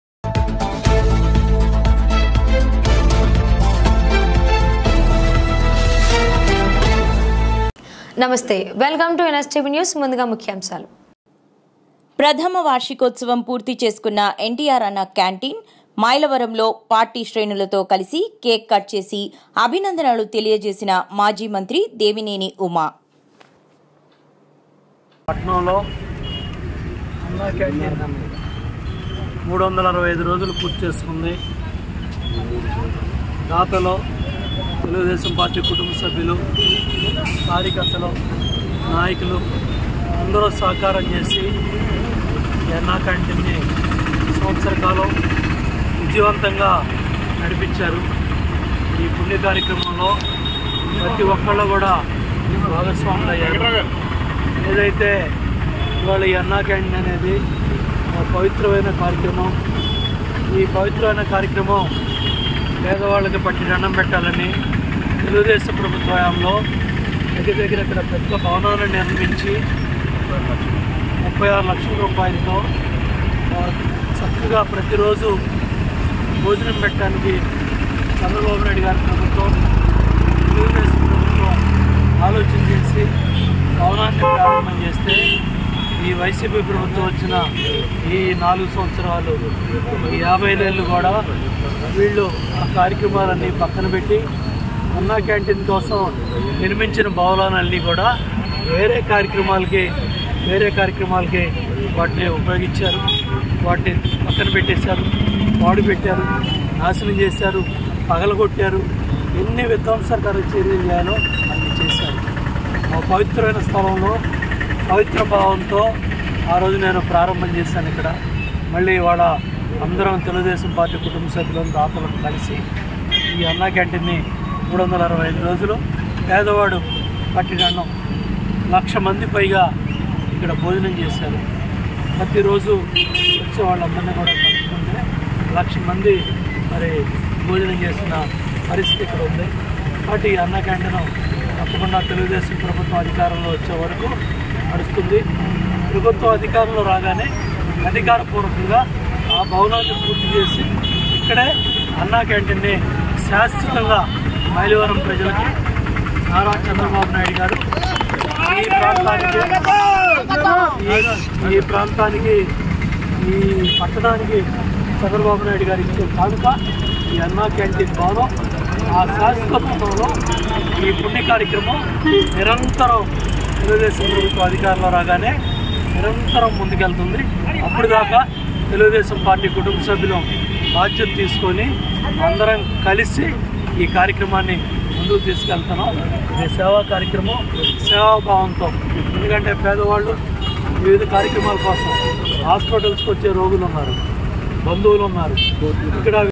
ప్రథమ వార్షికోత్సవం పూర్తి చేసుకున్న ఎన్టీఆర్ అన్న క్యాంటీన్..
ఎన్టీఆర్ జిల్లా
మైలవరం
మైలవరంలో పార్టీ శ్రేణులతో కలిసి కేక్ కట్ చేసి అభినందనలు తెలియజేసిన  మాజీ మంత్రి దేవినేని ఉమ